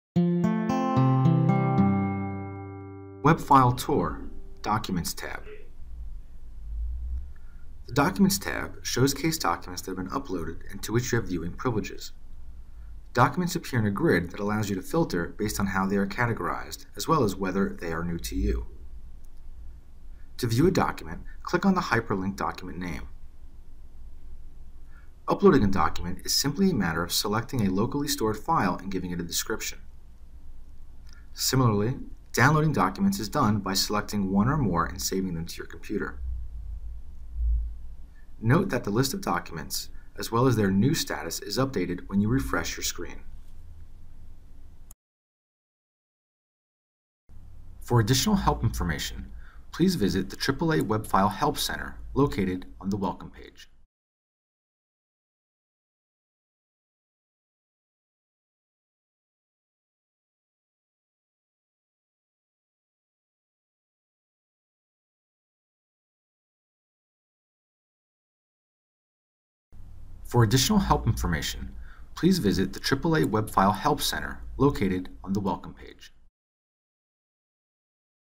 WebFile Tour: Documents Tab
The documents tab shows case documents that have been uploaded and to which you have viewing privileges.  The documents appear in a grid that allows you to filter based on how they are categorized as well as whether they are new to you.